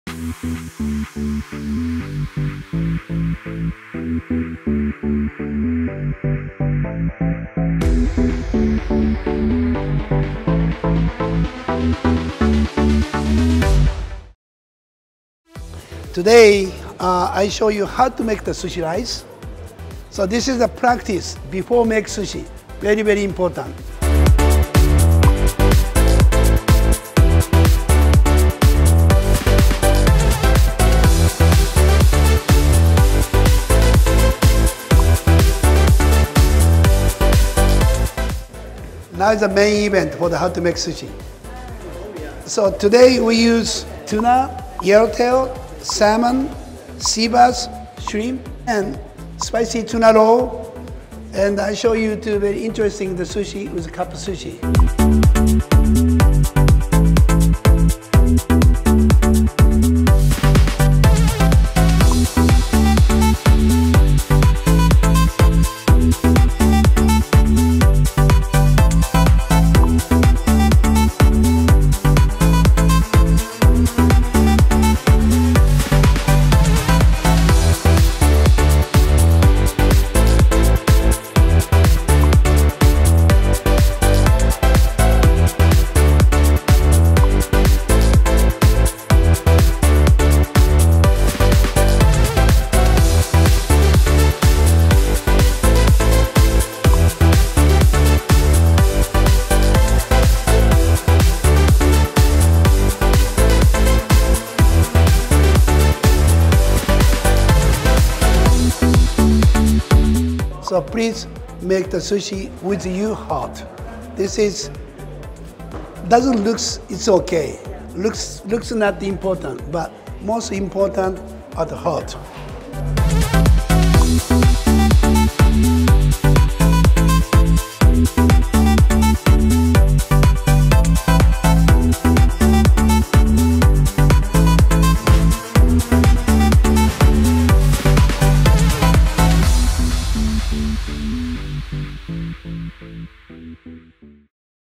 Sushi master-class with chef Nobu in Qatar
When it comes to sushi, there are few bigger names than Nobuyuki Matsuhisa, who is generally known as Nobu. The master Japanese chef has recently conducted a class at Four Seasons Hotel Doha to provide techniques for making perfect sushi. Be sure to watch the video to make your sushi experience more delicious.

Hamad Al Amari Snapchat: TheQatariGuy

Doha,
Qatar